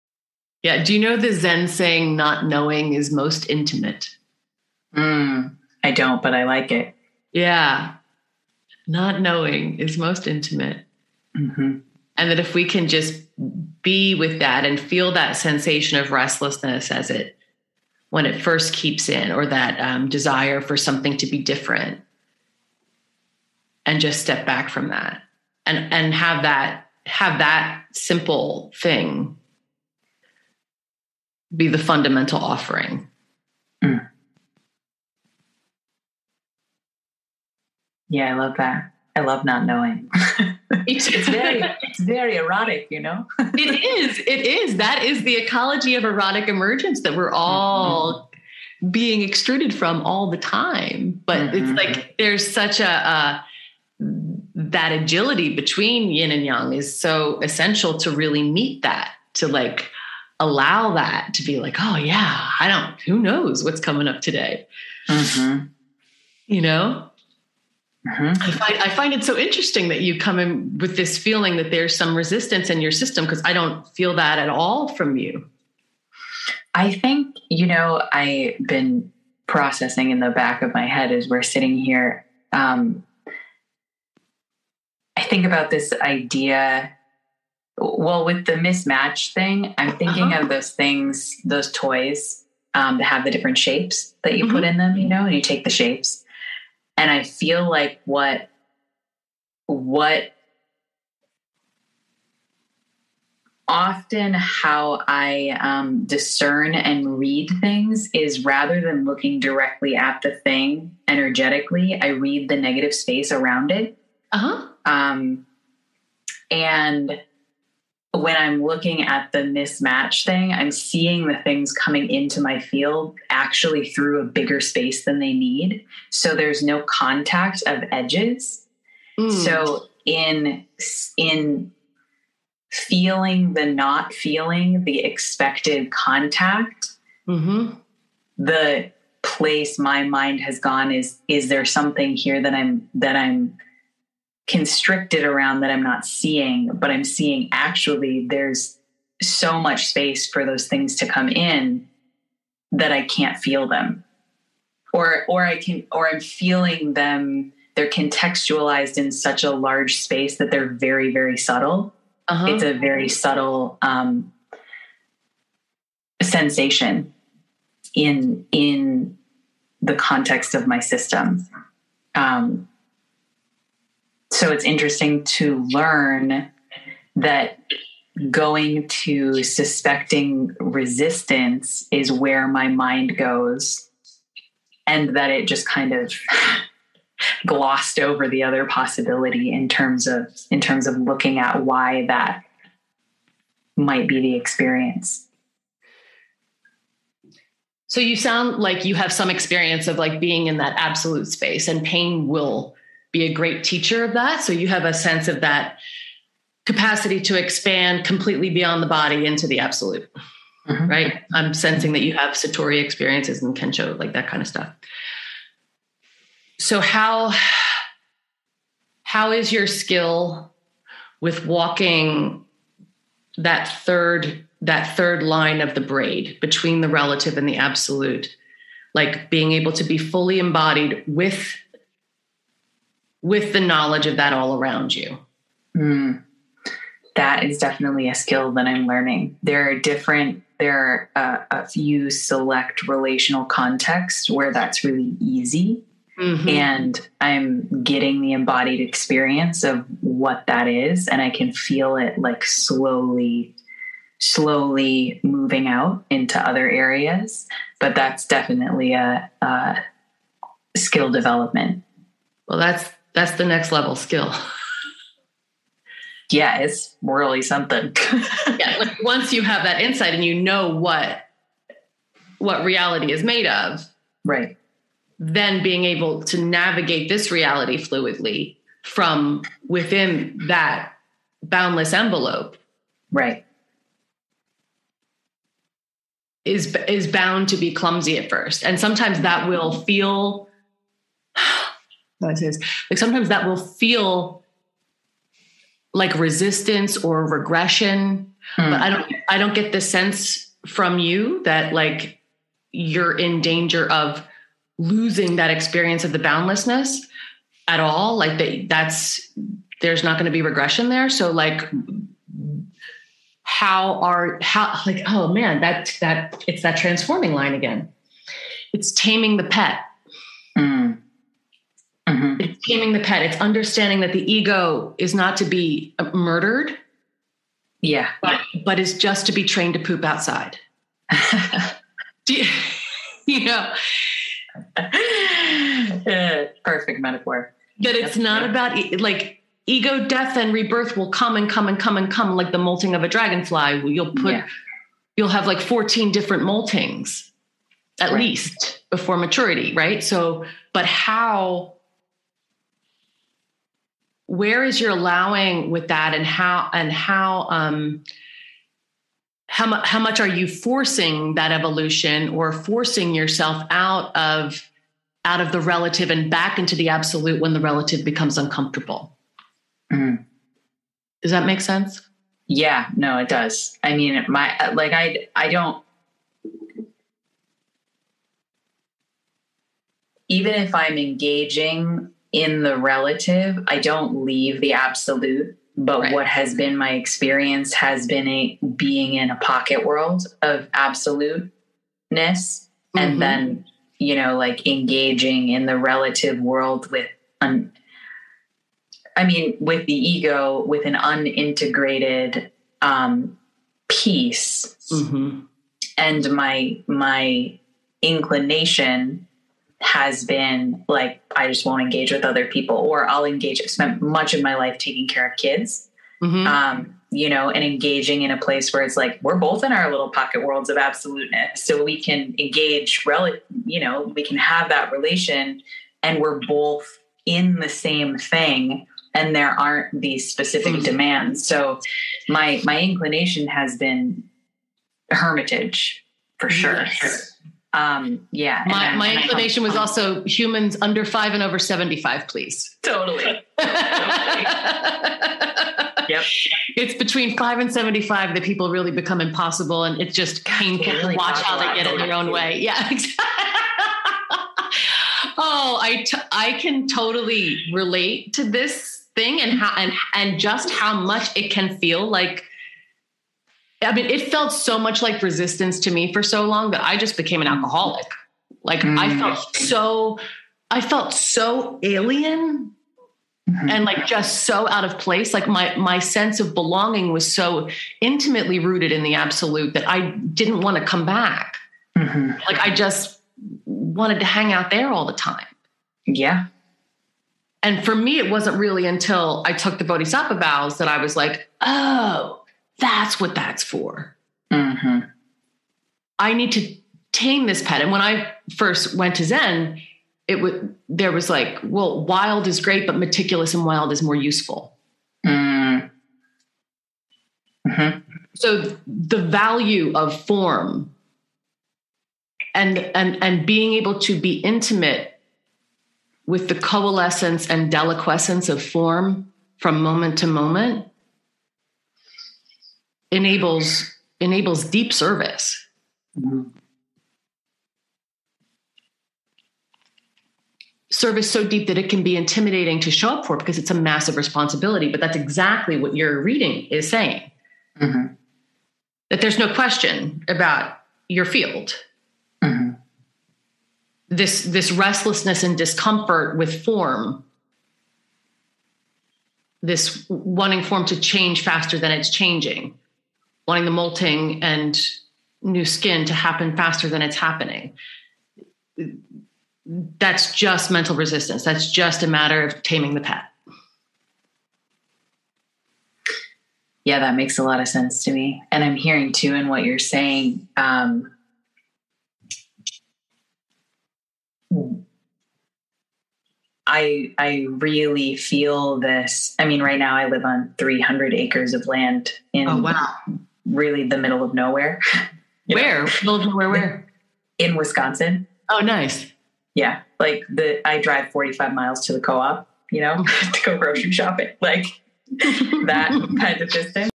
F*ck Suffering with Rei Hance Podcast Ep17 CLIP: How Can I Open to Miracles?
"Not knowing is most intimate," goes the Zen saying. And so often we resist that uncertainty and take that resistance for reality. This week's guest came in looking for a nudge from the I Ching around that.

I love this ep. Rarely do I have the joy of reading for someone at this point in her path, and when I do l am so grateful.

We get into the nuance of walking that third strand of the braid between the relative and absolute and how lit is the ease in that paradox. Not always comfortable, that ease. How do we embody it?